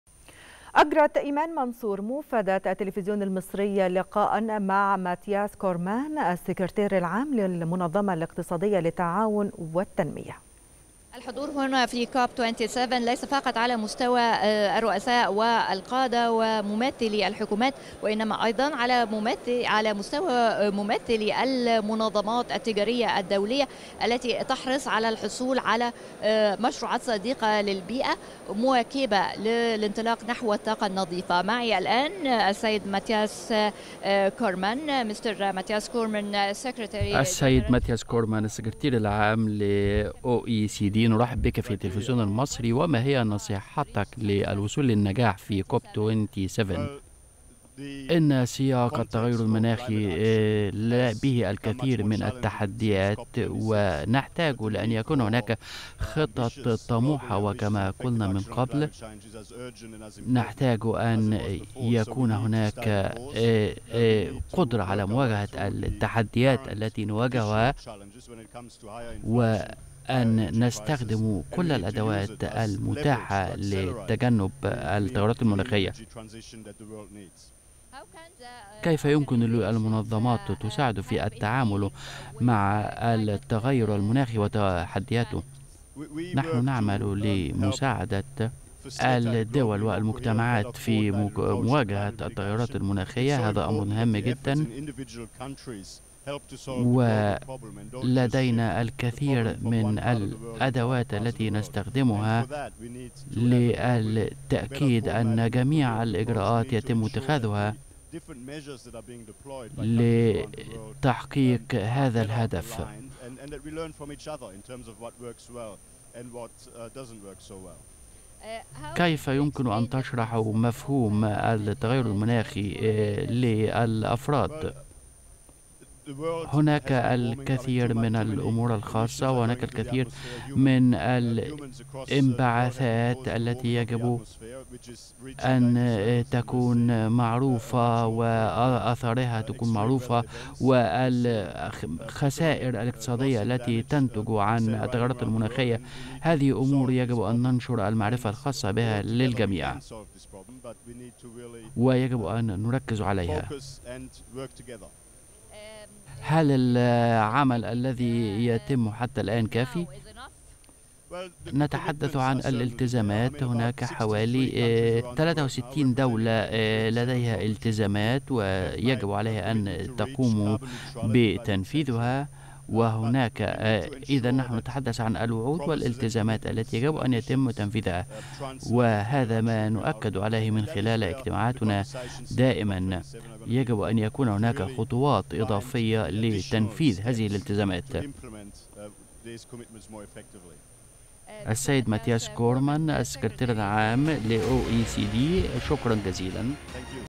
لقاء مع السكرتير العام للمنظمة الاقتصادية للتعاون والتنمية على هامش أعمال قمة المناخ كوب 27
لقاء مع ماتياس كورمان السكرتير العام للمنظمة الاقتصادية للتعاون والتنمية على هامش أعمال قمة المناخ كوب 27